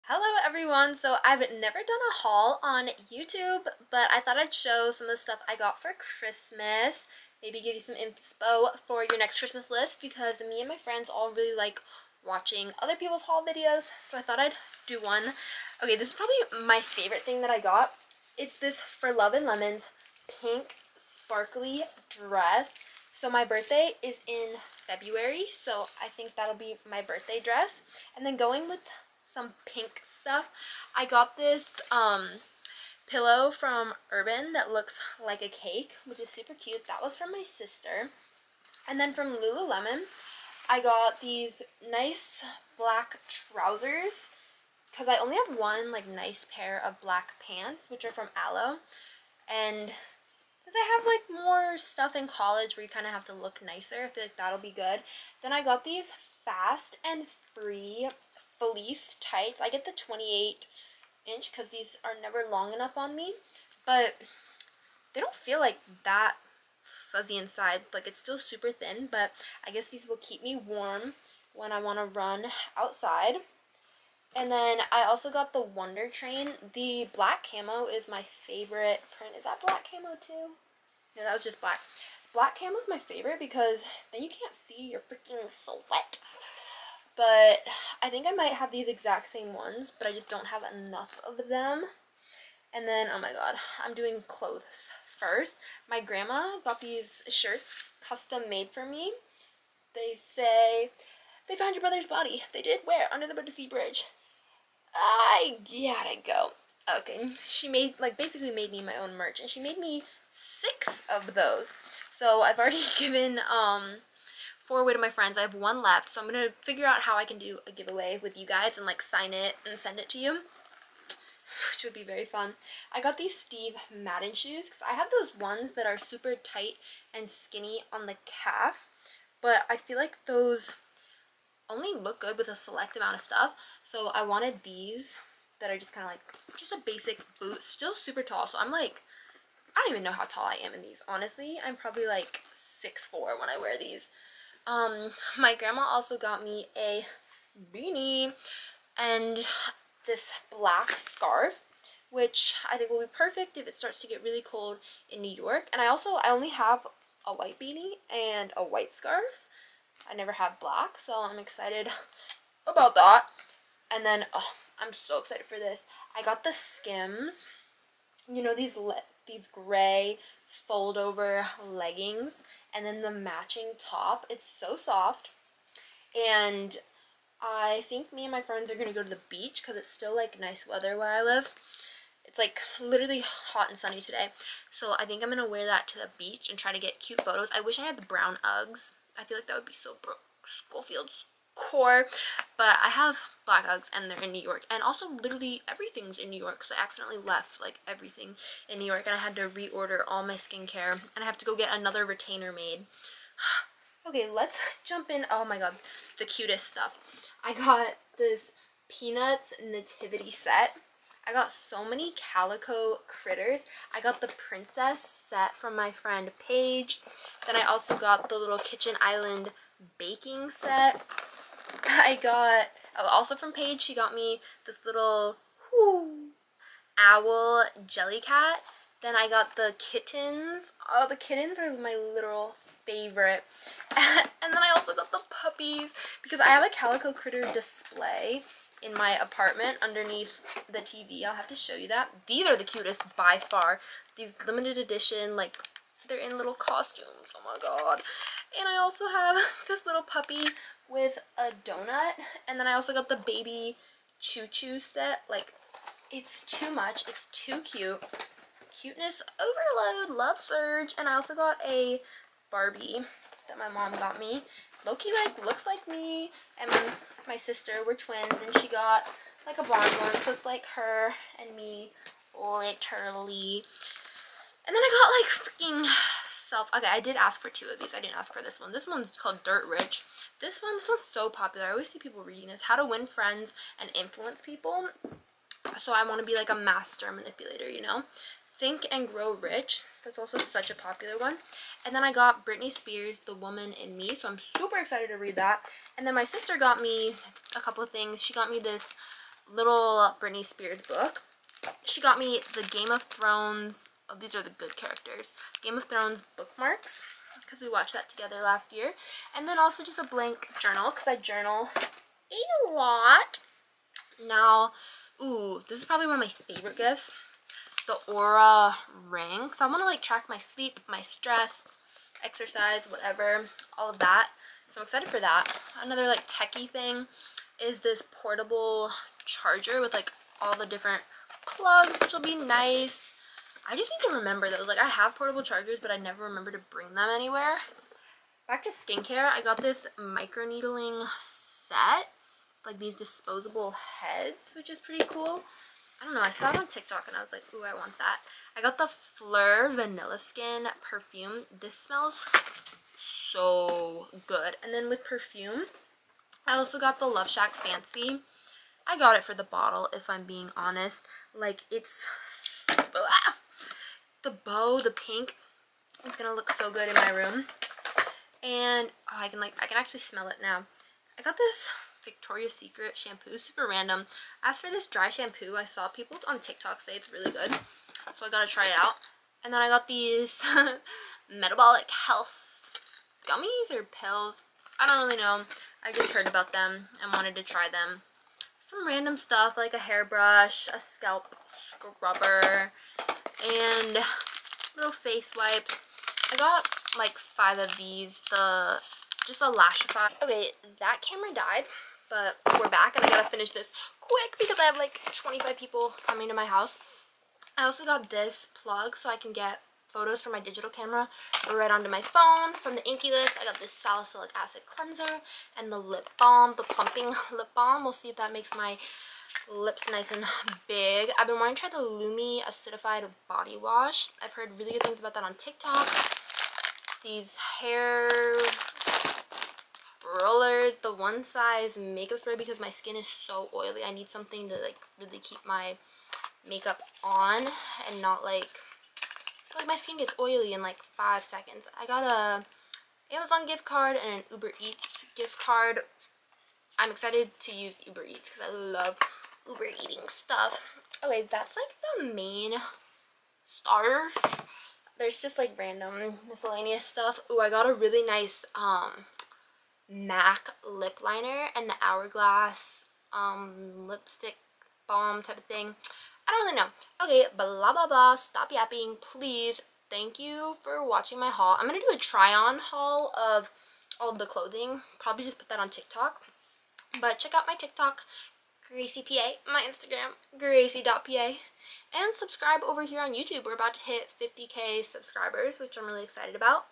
WHAT I GOT FOR CHRISTMAS 2023
HAULLLLL omg best Christmas everrr! Clothes, shoes, toys, books, and more. Calico critters were a big thing this year, I need to do a whole unboxing video. The pink For Love and Lemons dress will be perfect for my birthday. I need to start reading all of these books.

What did you get for Christmas? I want to know in the comments!